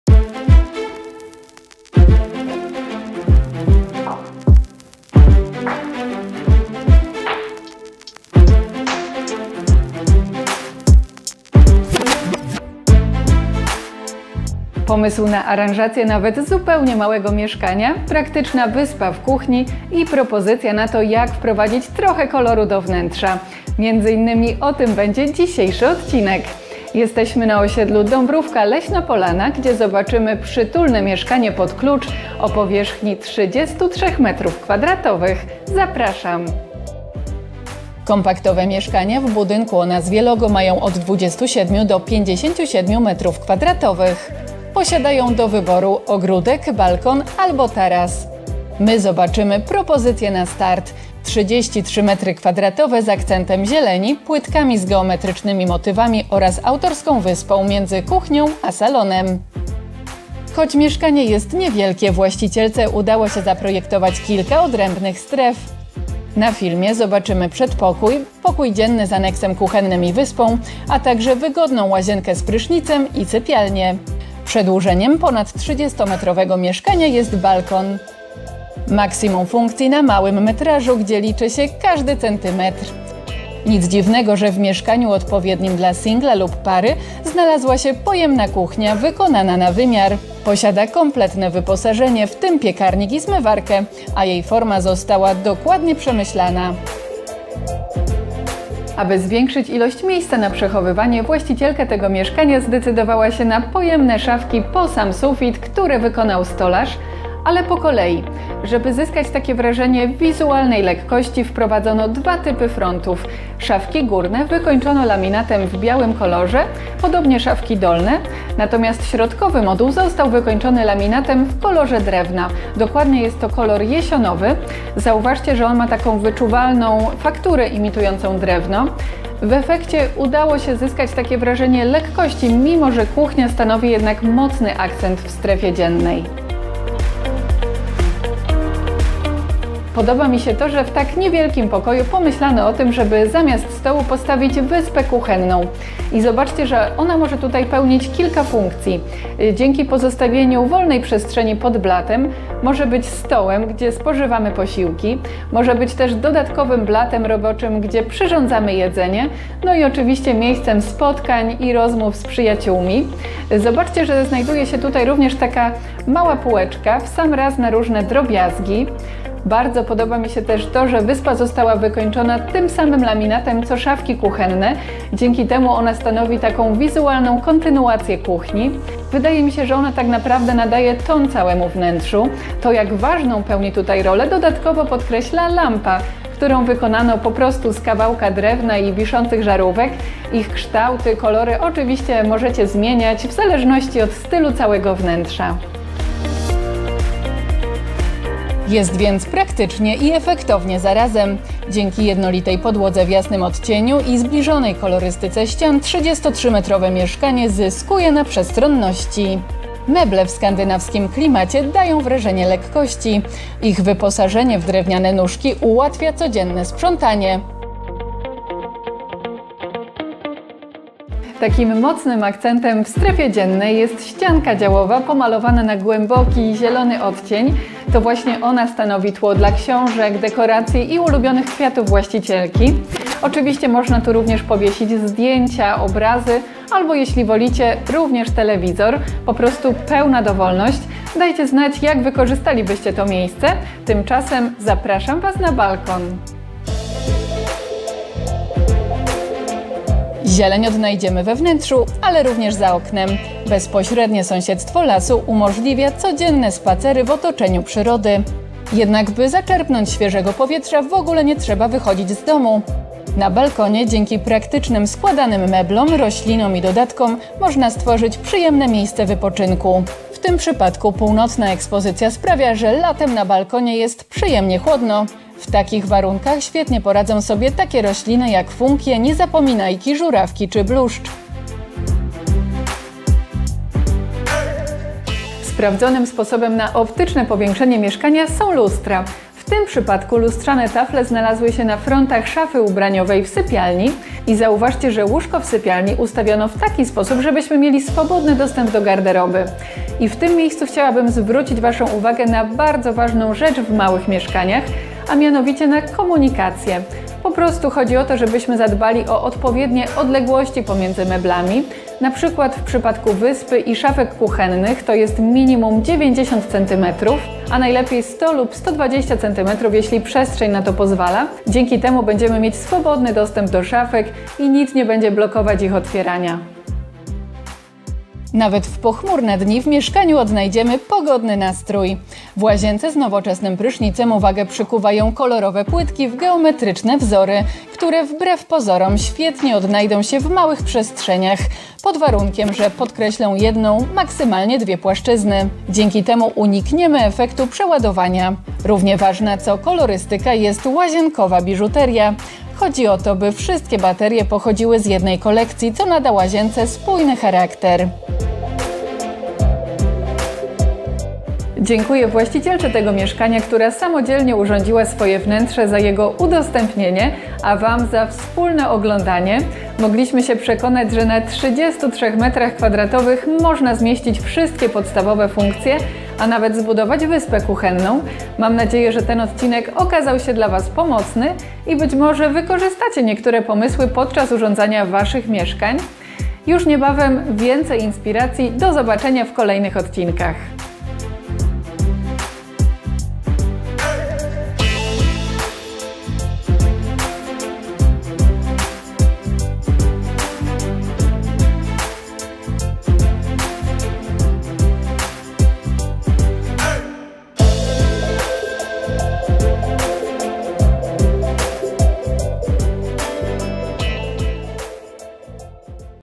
Zobacz autorską aranżację mieszkania 33 m2 z wyspą kuchenną! os. Dąbrówka - Leśna Polana w Dopiewcu
Wykończone mieszkanie w budynku typu LOGO! Zobacz jak można zaaranżować przestrzeń i na zaledwie 33 m2 zyskać funkcjonalny podział na strefy i praktyczne patenty do małej kuchni.
film produkcji @bliskopoznania.pl
Gdzie jesteśmy? Na osiedlu Dąbrówka - Leśna Polana pod Poznaniem,